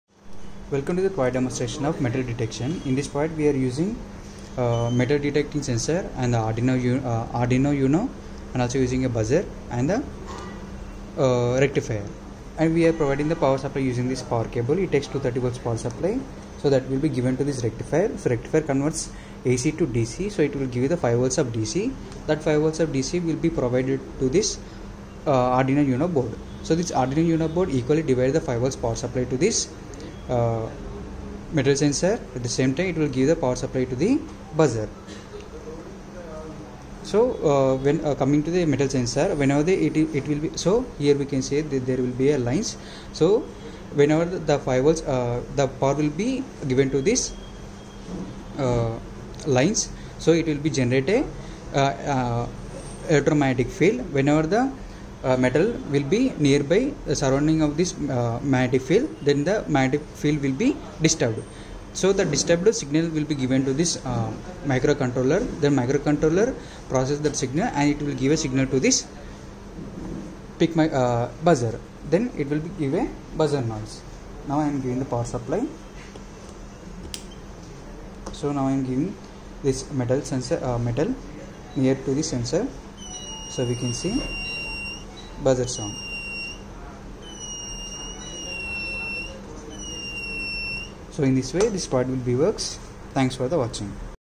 HVS-4302.Metal Detection using Arduino
ABSTRACT: Metal Detection using Arduino
The aim of the project is to construct a metal detector. The system is capable of alerting through buzzer in case of metals in its presence.
 The main controlling device of the project is Arduino. Metal sensor, buzzer is interfaced to the Arduino microcontroller. When the metal sensor detects the metal, this data process to the Arduino then Arduino will activate the buzzer for alerts. To achieve this task microcontroller loaded program written in embedded C language.

The features of the project are:
1. Construction of metal detector.
2. Alerts through buzzer.

The major building blocks of this system are:
1. Power Supply.
2. Arduino.
3. Buzzer with driver.
4. Metal sensor.